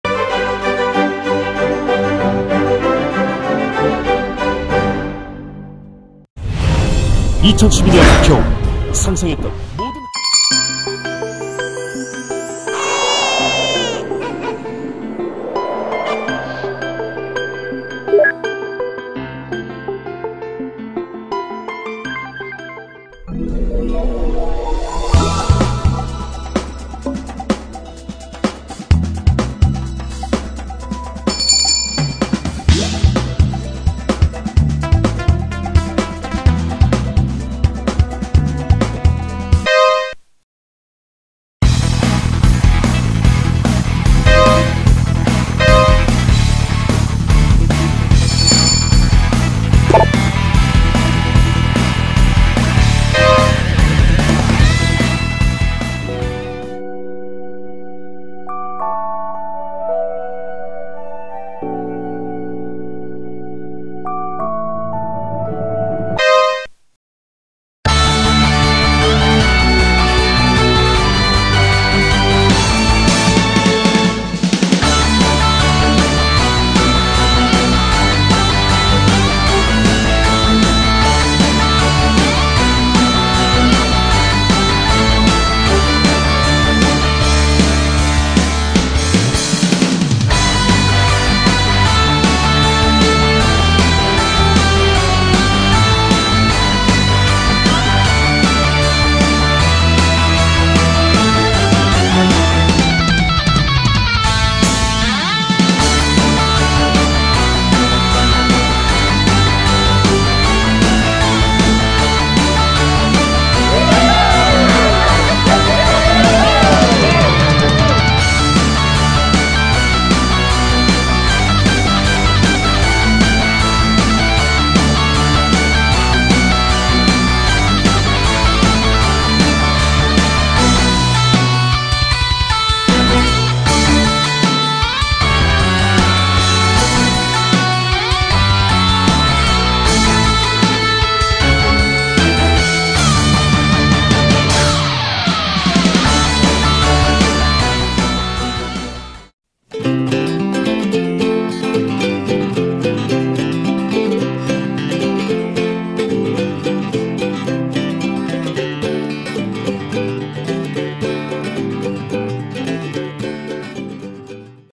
[한자] 메이플스토리 전섭최초 개인기록 2탄 영상과 관련하여 공지를 드립니다.(정말 레전드 영상 한편 나올 것 같습니다!) [한자플스토리]
하이하이 한자입니다~S2

너무 많은 분들이 문의를 계속해서 주시고 계셔서
관련사항들을 모두 알려드리겠습니다!!

진짜 진짜 저도 꿈만 같습니다!!!